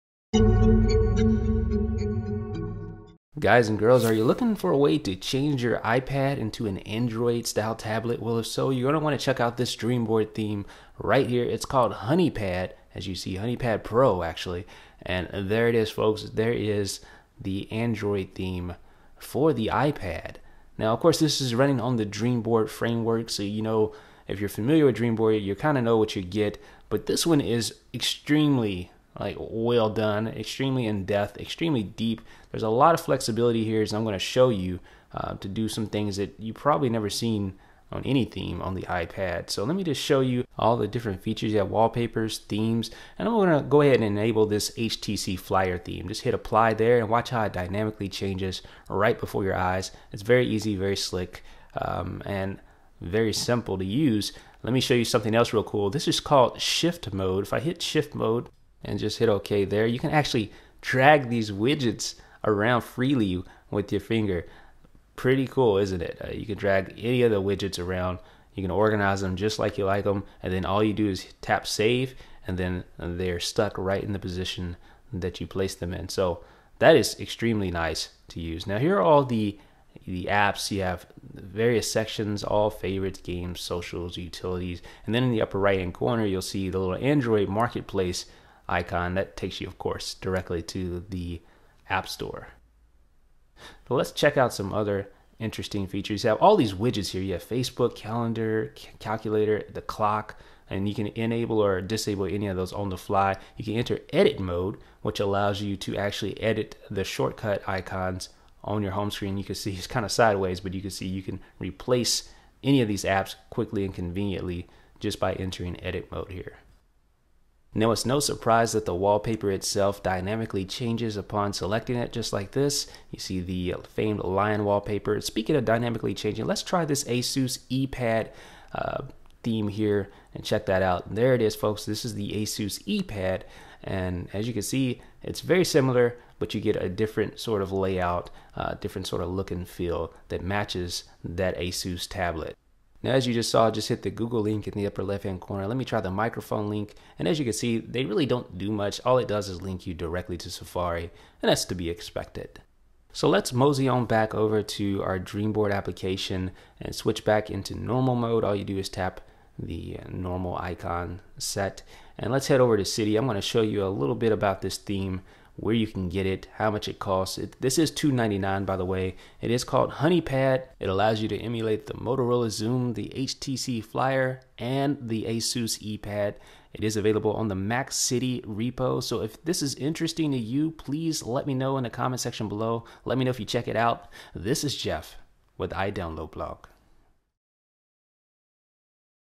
HoneyPad: How to Turn Your iPad Into an Android Tablet
Name: HoneyPad
Description: A DreamBoard theme that can alter your iPad to look like a Motorola Xoom, Asus Eee Pad Transformer, or HTC Flyer with just a few taps.
Price: $2.99
Repo: MacCiti
Developer: Roy Soetantio

About iDB